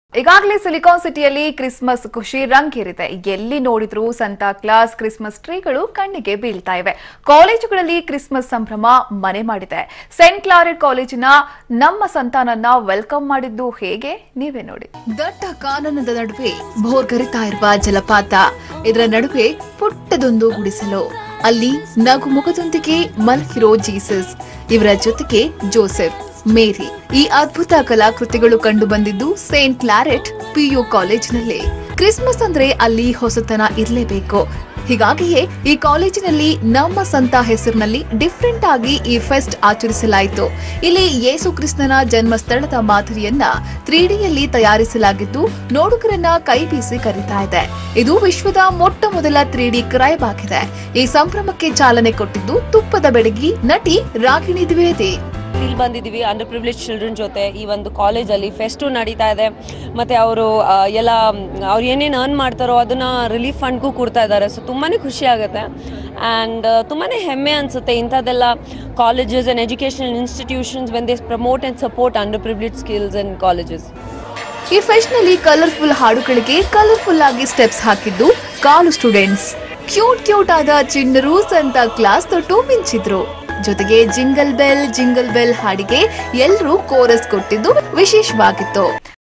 NAMMA SANTA ON PUBLIC TV | ST.CLARET PU COLLEGE, BANGALORE
Namma santa 2018 is an annual Christmas celebration organised by St.Claret PU College which brings together the underprivileged kids from all religious backgrounds  and fulfills one special wish of theirs. This edition saw Sandalwood actress Ragini Dwivedi and  Musician and Grammy award winner Ricky Kej as the chief guests.  We thank Public TV Bengaluru for the coverage of the event and their continued support